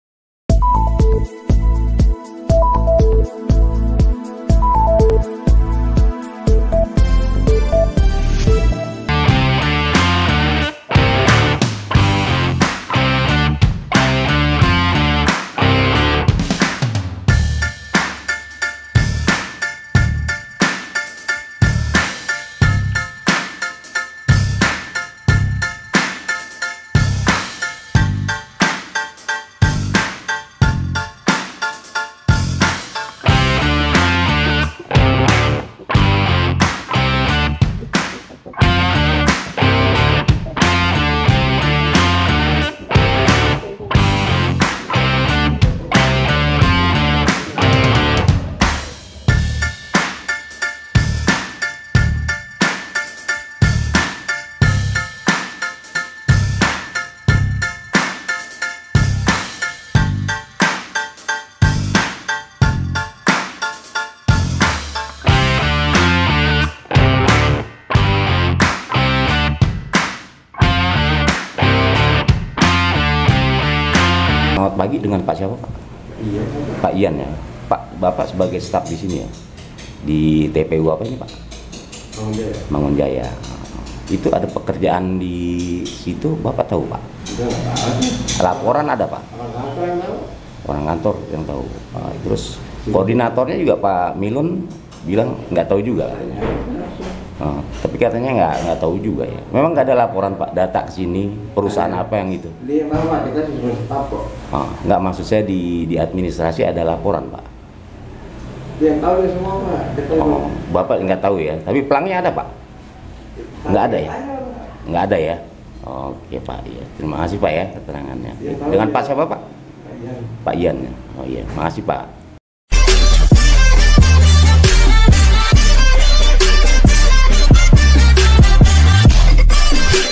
Proyek Pembangunan " Setan Kober " Muncul di TPU Mangun Jaya, Kabupaten Bekasi (MHI)
KABUPATEN BEKASI, MHI - Pekerjaan proyek pembangunan tanpa ada keterangan jelas dan diduga sarat akan tindak pidana korupsi kembali terjadi di Tempat Pemakaman Umum (TPU) Mangun Jaya, Desa Mangun Jaya, Kecamatan Tambun-Selatan, Kabupaten Bekasi, (8/12/2020).

Hal tersebut diketahui saat Tim Awak Media melintas di wilayah TPU tersebut, yang kemudian menyambangi lokasi pekerjaan proyek pembangunan yang berada tepat dipintu gerbang TPU Mangun Jaya.

Tim Awak Media menanyakan tentang pekerjaan proyek tersebut kepada para pekerja dilokasi tentang pekerjaan tersebut, anehnya mereka menjawab tidak tahu untuk apa dan mereka mengatakan," Saya tidak tahu..saya disuruh kerja ngaduk semen dan pasang batu-bata ..ya itu saja..kalau mau tahu tanya saja sama pak milun orang kantornya pak," Jawabnya dengan tidak mau menyebutkan namanya, (5/12/2020).

Tim Awak Mediapun bergegas ke Kantor TPU dan menjumpai Milun selaku Koordinator Kantor TPU Mangun Jaya, dan ketika ditanyakan perihal pekerjaan proyek tersebut, Milun mengatakan dengan nada tinggi," Wah kalo ditanya kerjaan itu mah saya kaga tau-tau..darimana kerjaannya dan siapa pemborongnya kaga tau-tau sayamah," Jawab Milun, saat ditanyakan tentang Papan Proyeknya, Milun jawab," Emang kaga ada papan proyeknya,"Jelasnya.

Tim pun menanyakan juga pada Iyan staff administrasi Kantor TPU, kemungkinan ada data laporan ke kantor terkait pekerjaan tersebut, namun saat ditanyakan, iyan mengatakan," Wah saya tidak tahu..tidak ada laporan," Katanya, ketika ditanyakan papan proyeknya, seraya Iyan menengok kelokasi dan mengatakan," Tidak ada pak," Jawabnya singkat.

Kemudian Tim Awak Media bertemu dengan warga sekitar yang tinggal diwilayah itu di depan TPU Mangun Jaya, mereka Ridwan, Indra dan lainnya memberikan keterangan pada Tim Awak Media terkait pekerjaan pembangunan  tersebut mengatakan," Itu kerjaan kaga tau dah...orang tau-tau udah ada kerjaan gitu dah...masyarakat juga banyak yang komplain..plang kerjaannya juga kaga ada..itu tau kerjaan dari mana itu ..apa dari medi atau apa tau itu..itumah kayaknya kerjaan Setan Kober..orang tau-tau udah ada kerjaan begitu bae..ya ..namanya kerjaan Setan Kober..orang kerjaan tanem apa itu tau," Terang mereka pada Awak Media.

"Orang pernah ditanyain sama orang kantornya..ora ada yang mau ngaku..kalo ditanya sono orang sono..orang sono..lha tau dah..jadi ora jelas..jadi namanya kerjaan Setan Kober,"Imbuhnya seraya menutup pembicaraan.

Salah satu dari mereka menghubungi Ketua RT setempat melalui Whatsapp dan menanyakan tentang pekerjaan proyek tersebut, namun jawaban yang sama didapati dari SMS Whatsapp Ketua RTpun menjawab tidak tahu dan tidak ada laporan. (MHI).